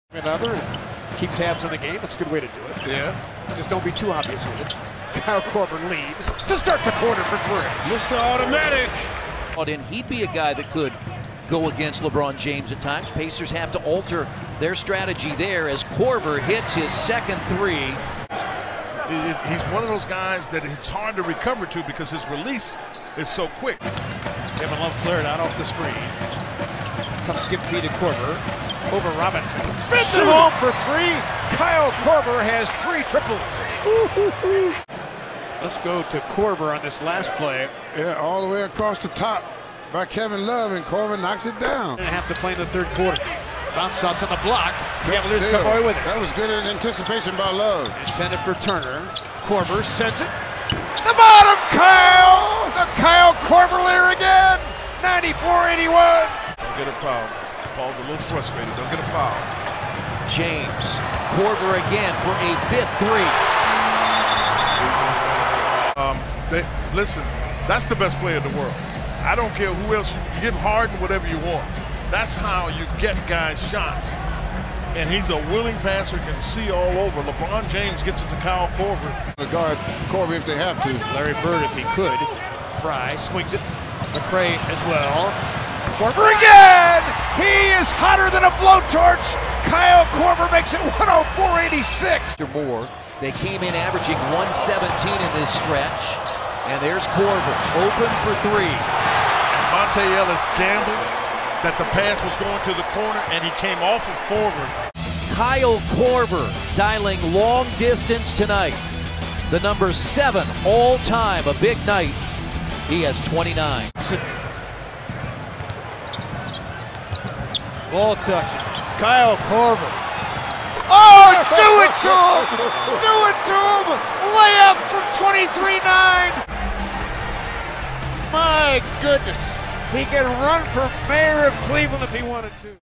Kyle Korver's 8 Made 3-Pointers, Most Since 2007
Kyle Korver knocked down eight 3-pointers tonight a win against the Pacers. This was Korver's best performance from downtown since 2007!